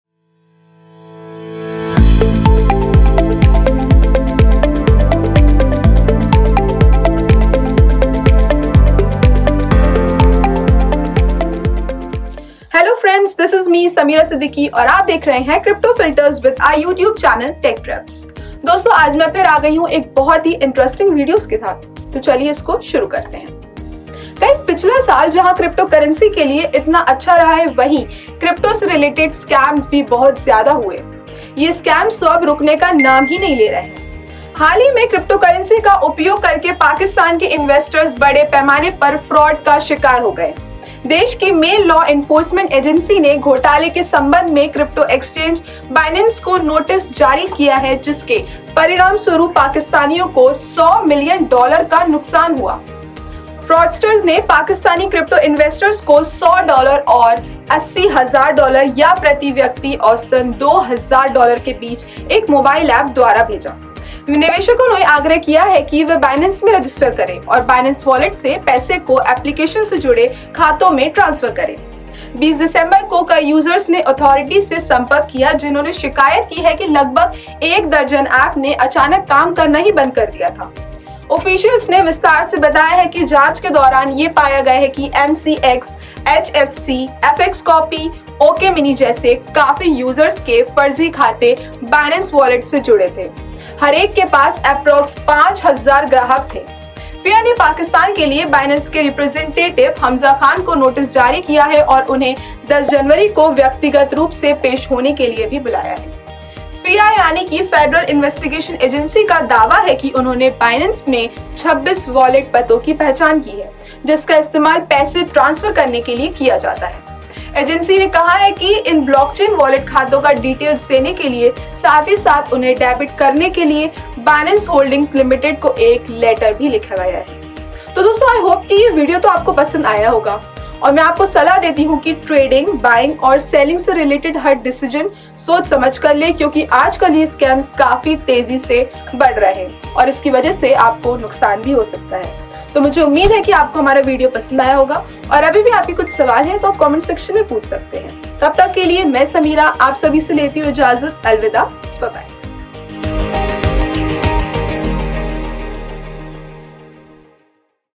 Pakistani Crypto users got Robbed by scammers through Binance platform CryptoFilters EP96
Pakistan issues notice to Binance

Pichhla saal jaha cryptocurrency ke liye itna accha raha vahi crypto se relared scams bhi bohot jyada hue… yeh scams to ab bhi rukne ka naam nahi le rahe hain..
Haal hi me क्रिप्टोक्यूरेंसी का उपयोग करके पाकिस्तान के investors बड़े पैमाने पर fraud का शिकार हो गए हैं। देश की main law enforcement agency ने घोटाले के संबंध में क्रिप्टो एक्सचेंज बिनेंस को नोटिस जारी किया है जिसके परिणामस्वरूप पाकिस्तानियों को $ 100 मिलियन डॉलर का नुकसान हुआ है |
fraudsters ने पाकिस्तानी क्रिप्टो investors को $ 100 और $ 80,000, या प्रति व्यक्ति औसतन $ 2,000 के बीच ek mobile app dwara भेजा। निवेशकों से आग्रह किया गया कि वे बिनेंस में register करें, और बिनेंस वॉलेट से पैसे को एप्लिकेशन से जुड़े खातों में transfer करें। 20 दिसंबर को, कई users ने authorities से संपर्क किया, जिन्होंने शिकायत की कि लगभग एक दर्जन ऐप ने अचानक काम करना बंद कर दिया था।
officials ने विस्तार से बताया ki जांच के दौरान, यह पाया गया कि MCX, HFC, HTFOX, FXCOPY, OKMINI, BB001, AVG86C, BX66, 91fp, TASKTOK, जैसे KAAFI USERS के फर्जी खाते बिनेंस वॉलेट से जुड़े थे।" HAR EK के पास APPROX 5,000 ग्राहक थे। FIA ने पाकिस्तान के लिए बिनेंस के representative हमजा खान को नोटिस जारी किया है और उन्हें 10 जनवरी को व्यक्तिगत रूप से पेश होने के लिए बुलाया है।
FIA Federal Investigation Agency का दावा है कि unhone बिनेंस में 26 वॉलेट पतों की पहचान की है जिसका इस्तेमाल पैसे ट्रांसफर करने के लिए किया जाता है। एजेंसी ने कहा ki इन ब्लॉकचैन वॉलेट खातों का details देने के साथ-साथ उन्हें डेबिट करने के लिए बिनेंस होल्डिंग्स लिमिटेड को एक letter लिखा गया है,"

Other Relevant Episodes
Content By: Aanchal Valecha
Anchor: Samira Siddiqui
Video: Sanjeev
Production: Tarang Global Studios, a unit of Tarang Global School of Arts